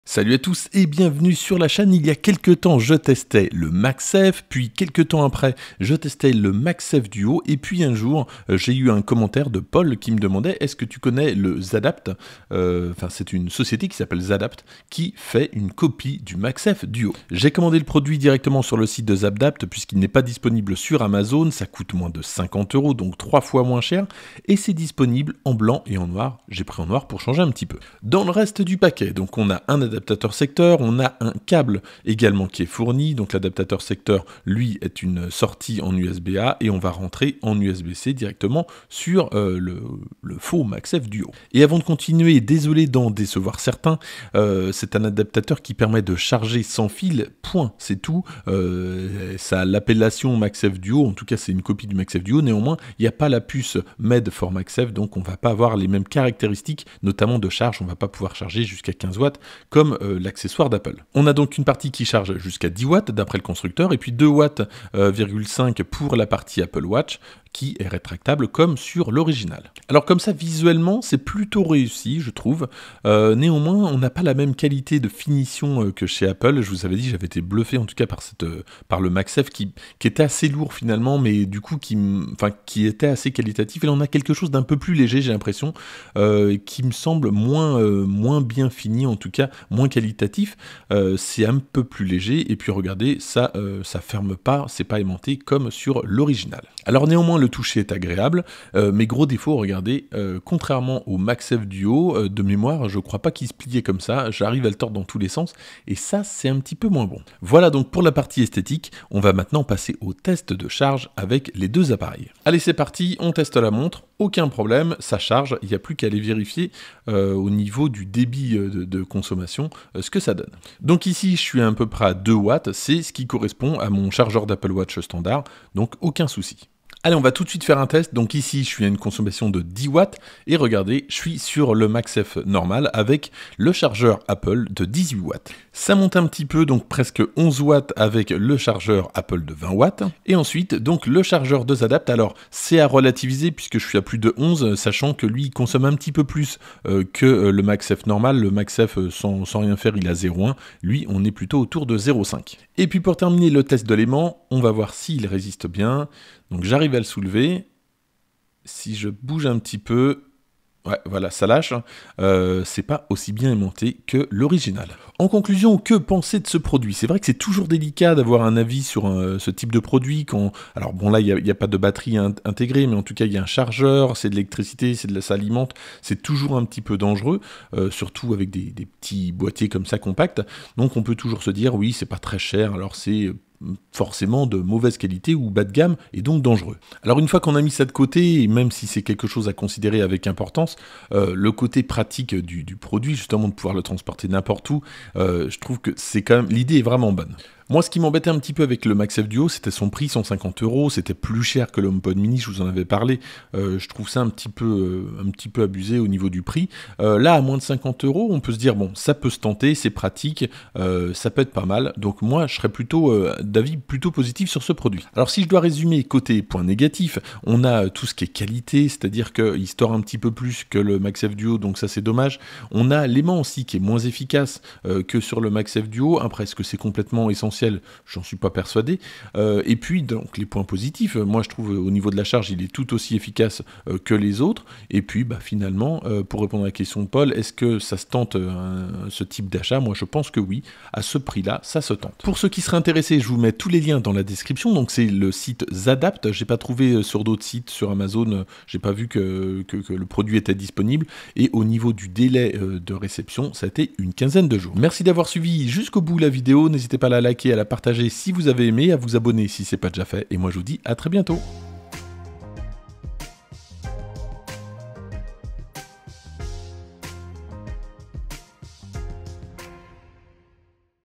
Un "Magsafe DUO" à moins de 50 euros ? Je teste ce modèle de ZADAPT
Dans cette vidéo, je teste une copie de MagSafe DUO à moins de 50 euros du fabricant ZADAPT.
Je vous donne mon avis sur ce produit .
Achetez sur le site de ZADAPT + Code de 10% : AMAZINGMAC10